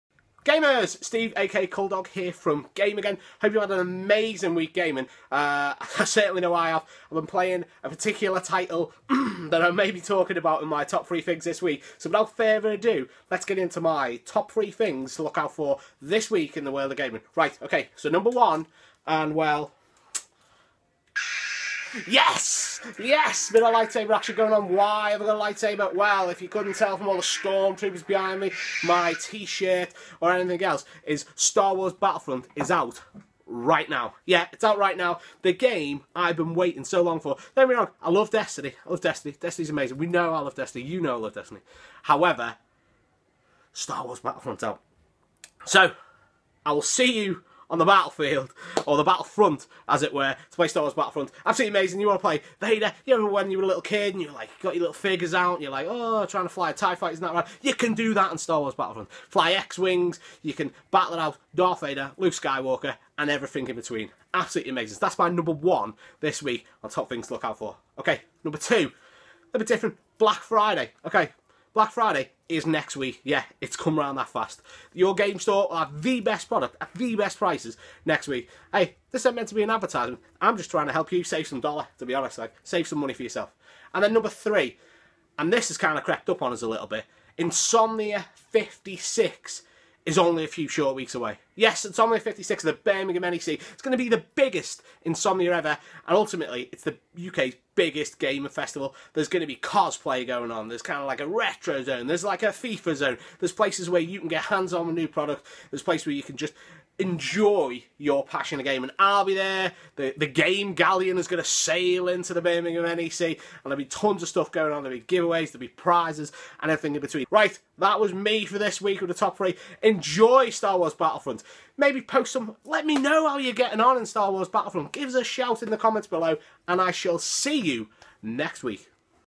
GAME Talks: Steve's Top 3 (20/11)
This week, we had to FORCE Steve to get off EA Access and tell us what the top three things in his galaxy of gaming are.